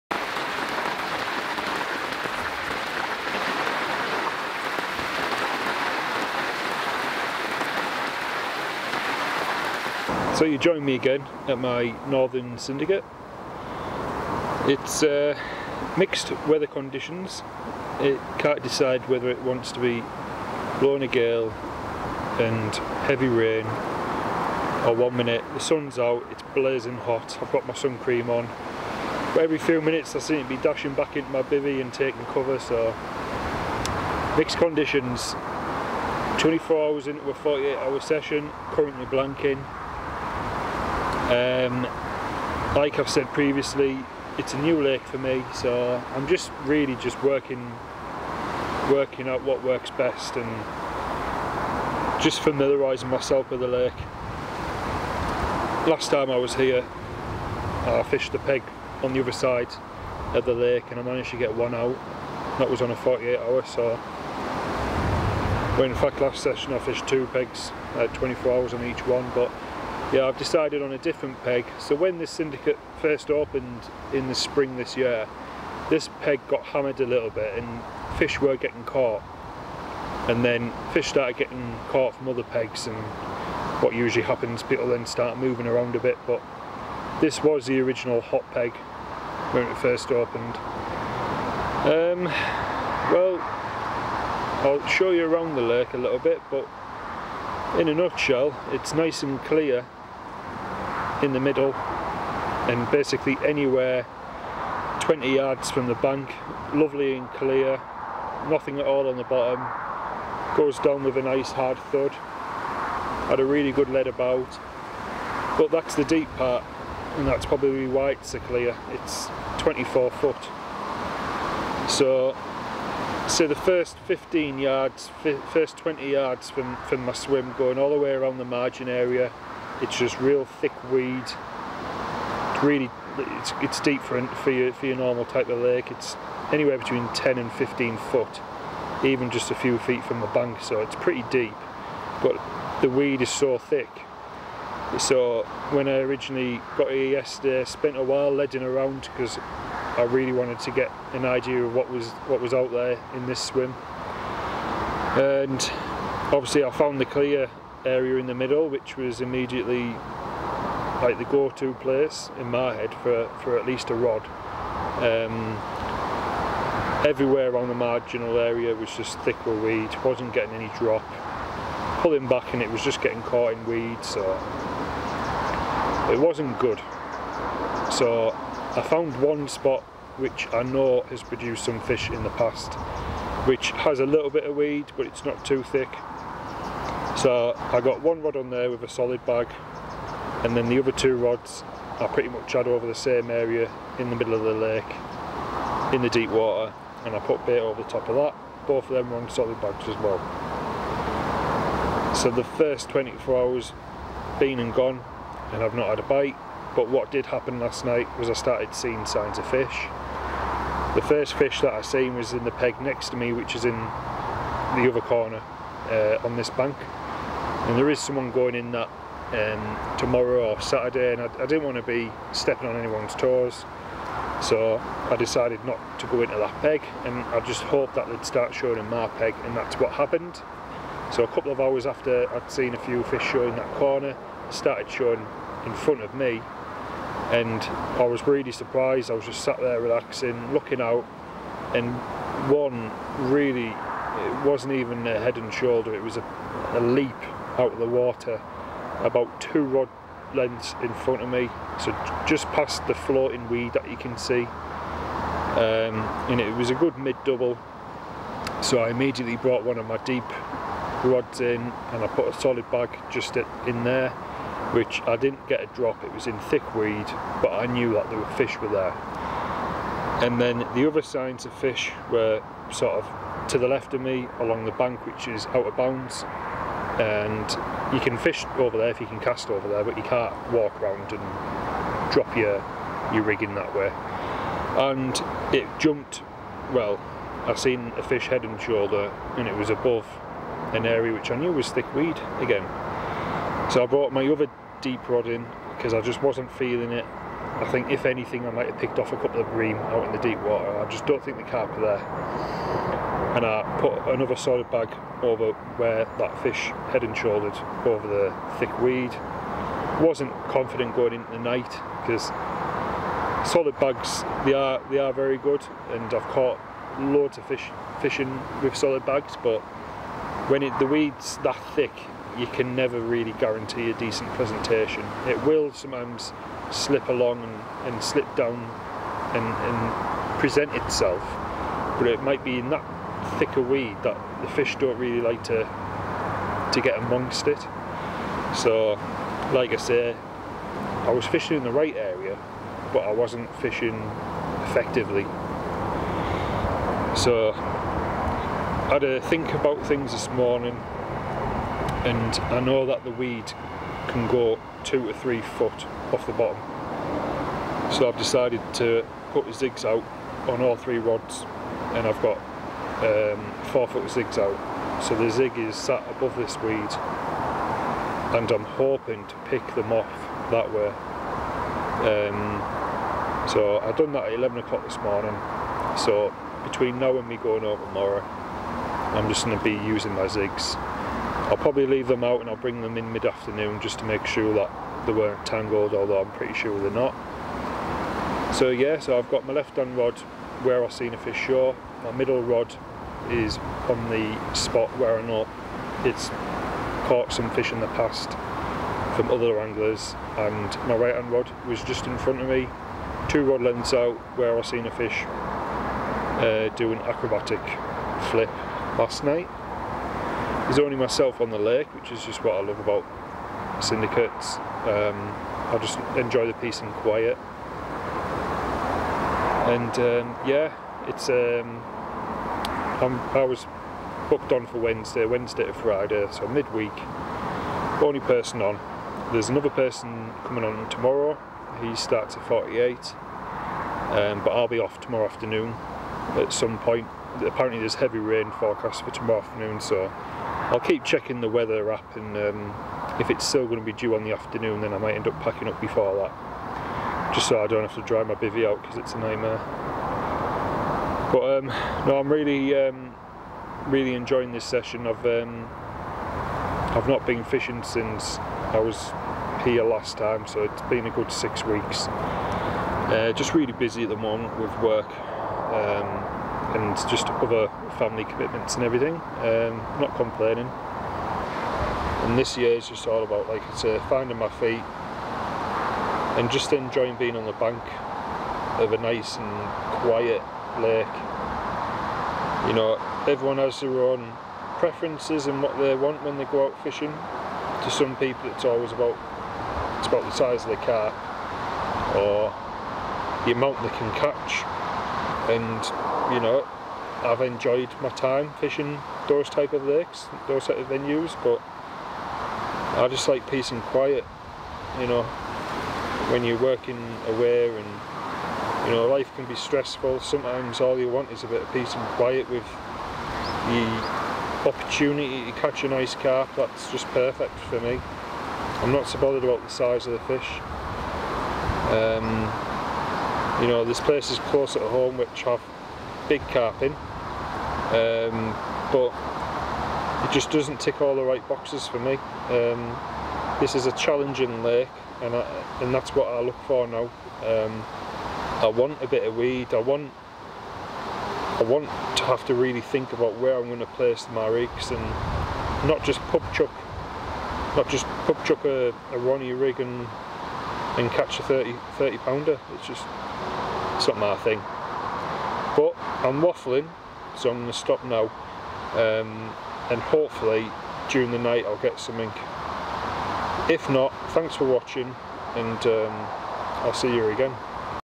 Local syndicate 2nd Visit (July 2021)
Join me on only my 2nd visit to my new syndicate water.
Met with mixed weather conditions, and having to have a good lead around to find potential spots to fish in.